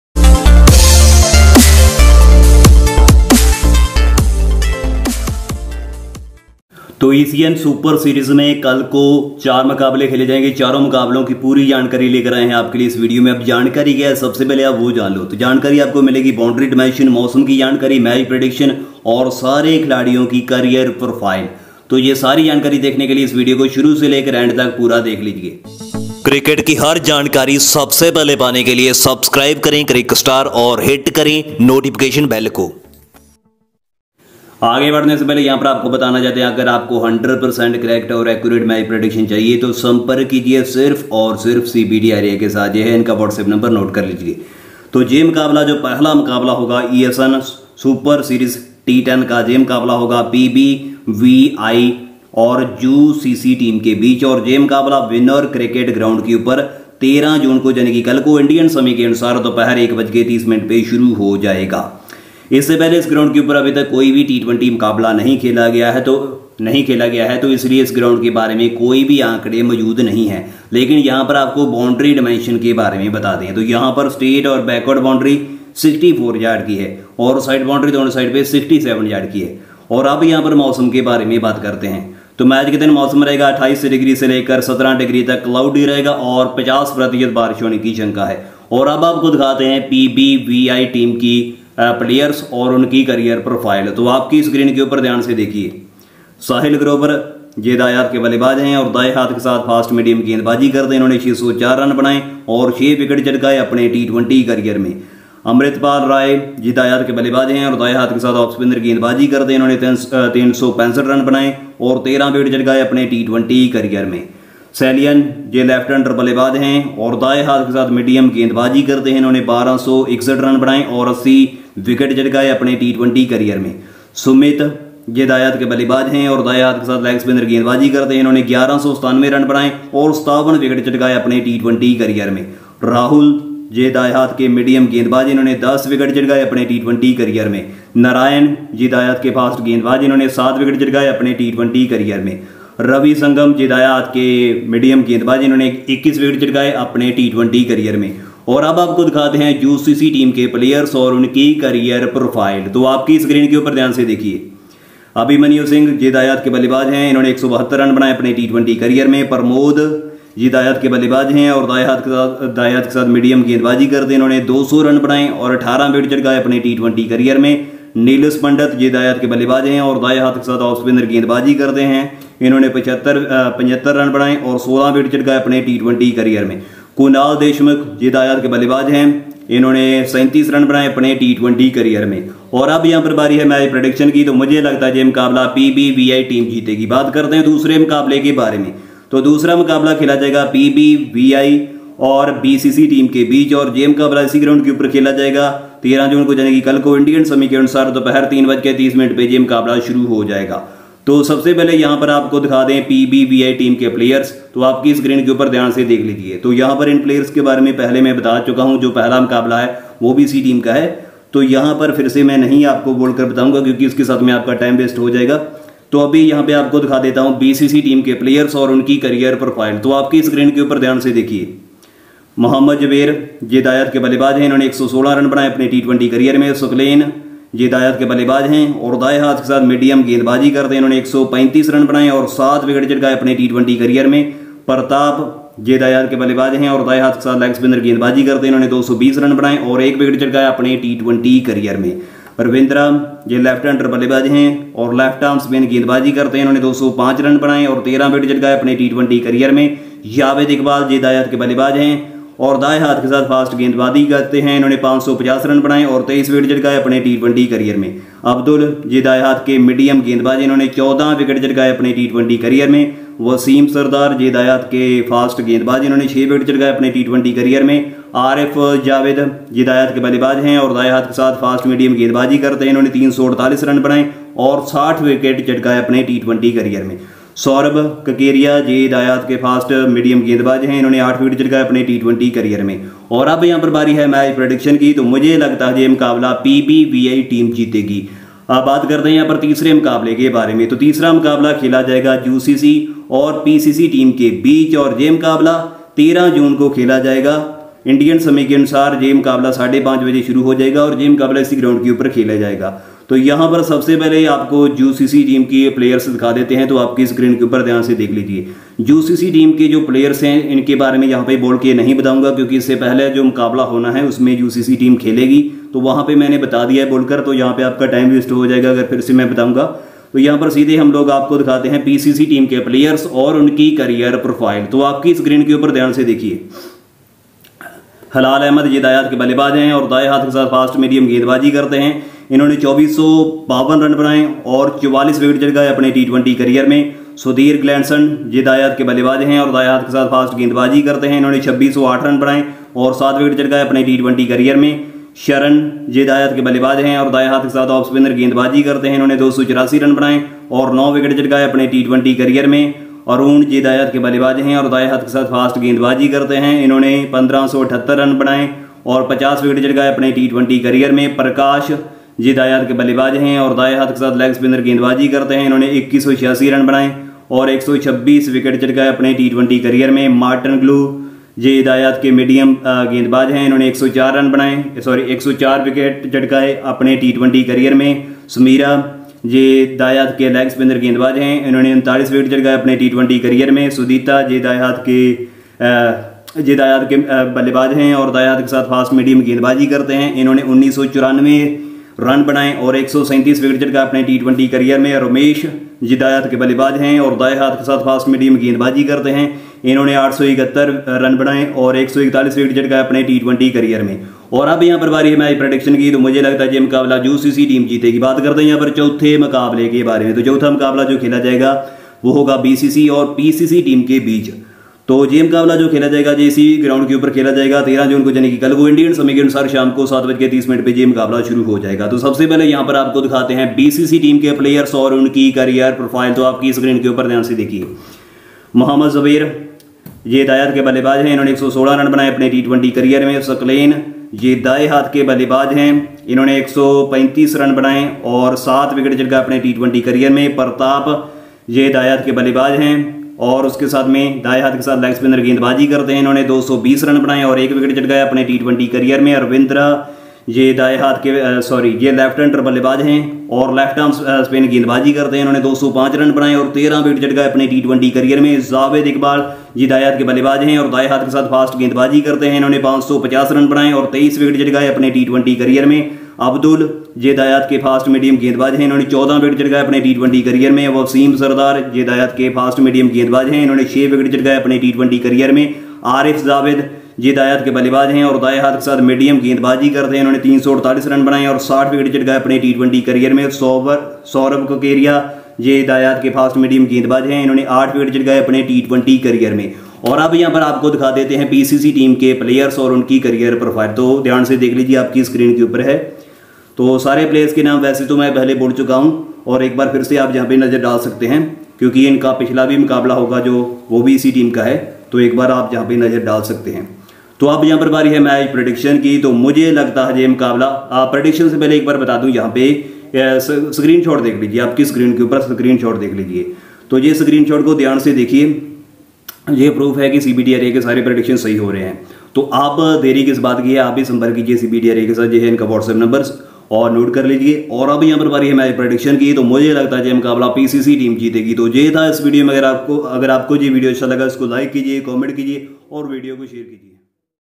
ECN Czech Super Series T10 | PBVI vs UCC | PBVI vs BCC | UCC vs PCC
in this video i told about ecn czech super series t10 2020 match 1 prague barbarians visigoths vs united cricket club match 2 prague barbarians visigoths vs bohemian cricket club match 3 united cricket club vs prague kings cricket club and match 4 bohemian cricket club vs prague kings cricket club will be played at vinor cricket ground i told about pitch report, weather update, playing 11, best batsmen & bowlers for dream11 team and match prediction i hope you like this video